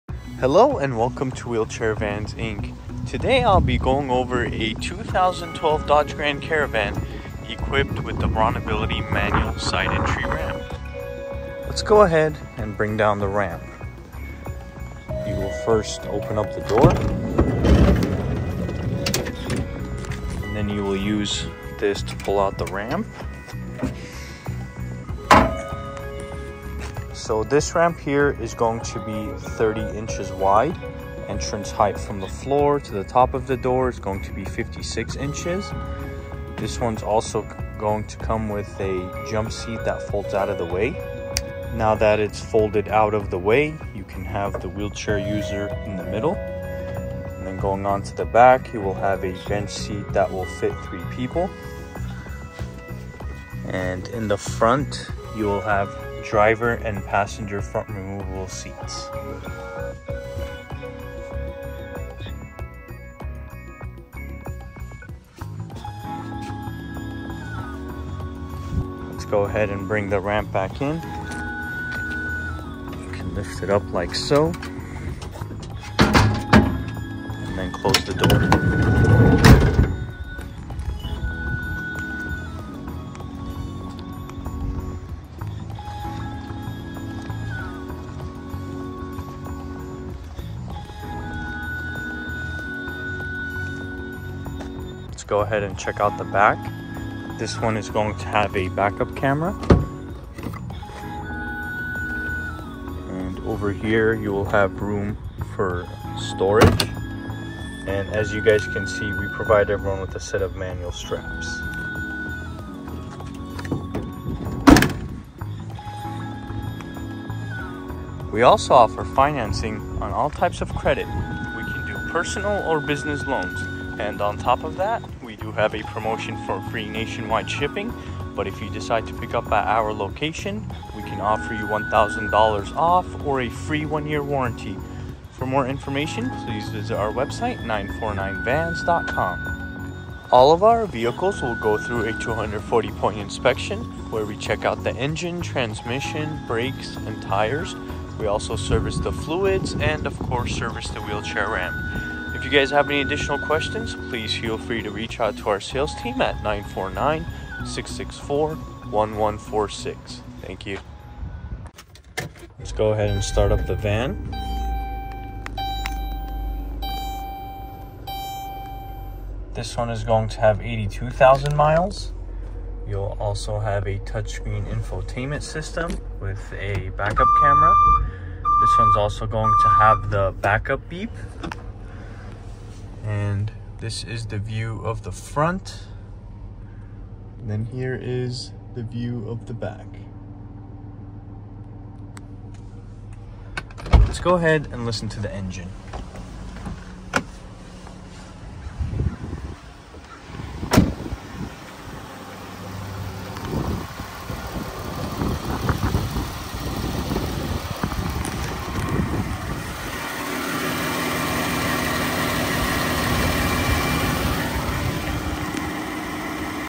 For Sale 2012 Dodge Grand Caravan BraunAbility XT Manual Fold Out Ramp Side Loading Wheelchair Van
Sale priced $26,995 with Free Nationwide Shipping For Sale 2012 Dodge Grand Caravan BraunAbility XT Manual Fold Out Ramp Side Loading Wheelchair Van